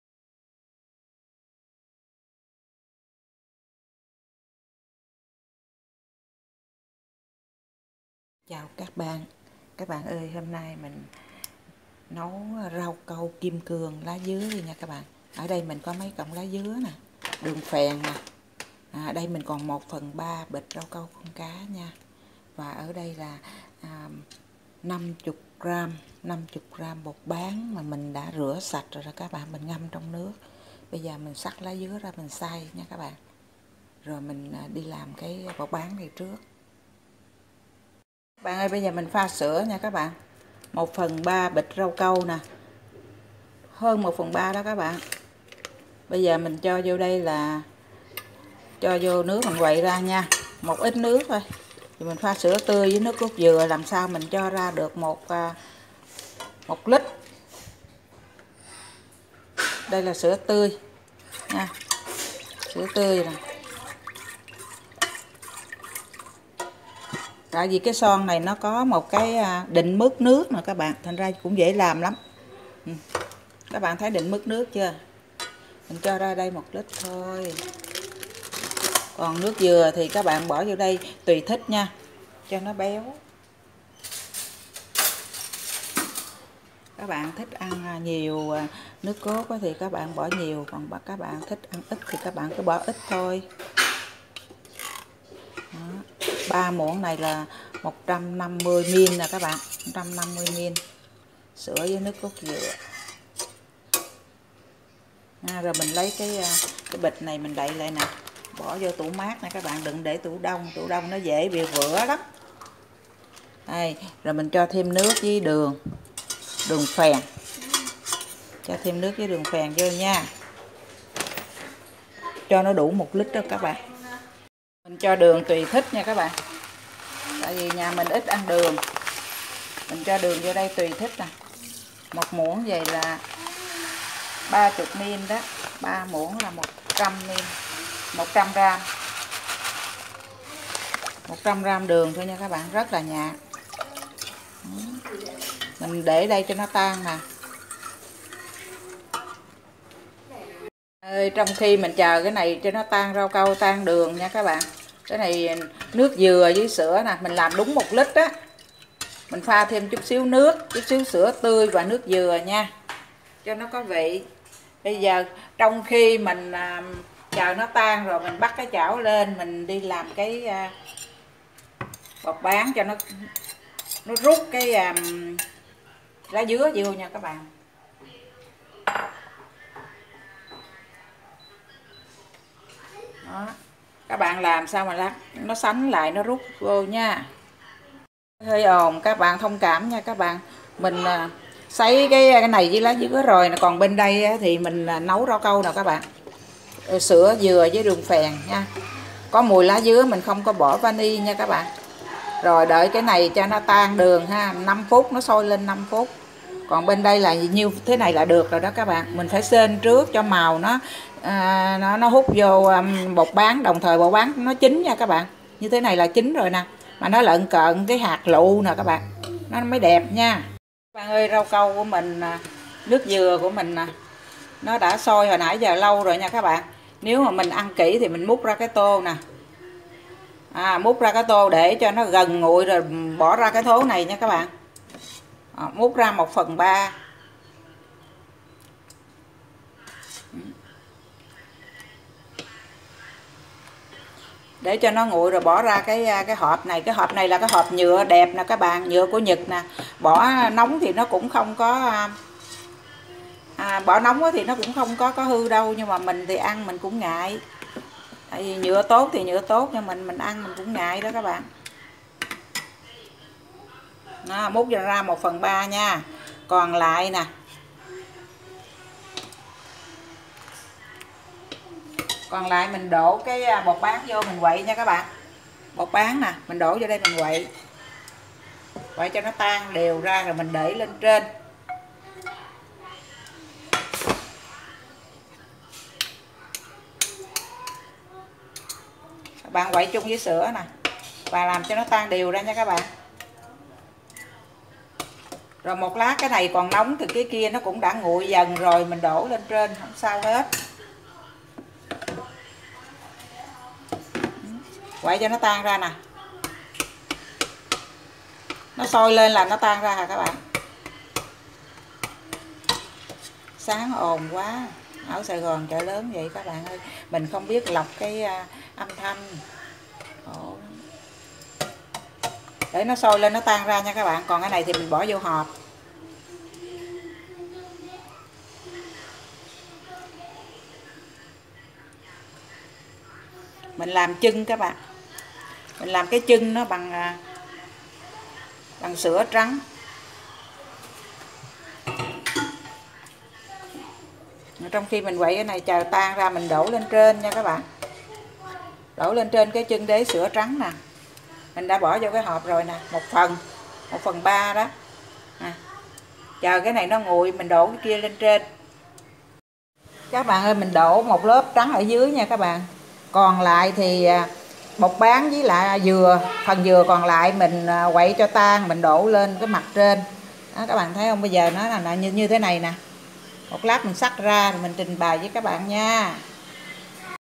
RAU CÂU DỨA SỮA Giải nhiệt ngày nắng nóng .
Rau câu .cốt dừa .cốt dứa .đường phèn .ít bột bán.sữa tươi..